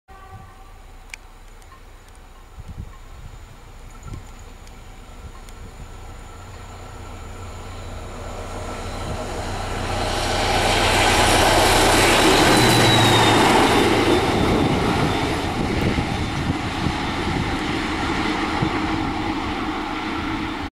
SM42-1148 Luzem
Film przedstawia przelot wyluzowanej zielonej stonki w stronę Mławy.